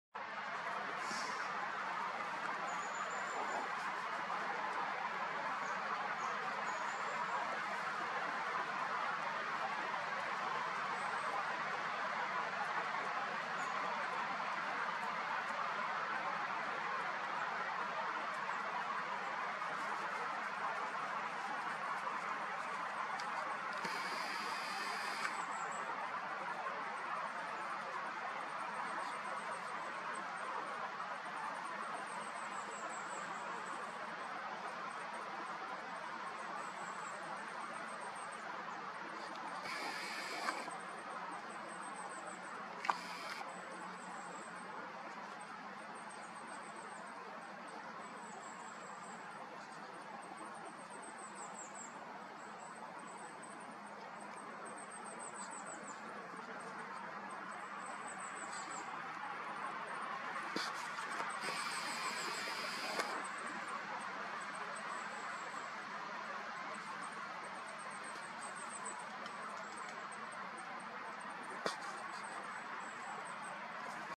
Corvus bistro & hide & Garrulus bistro 191031k028
Während die Krähe ihre Beute "versteckt", holt sich der Häger sein Teil.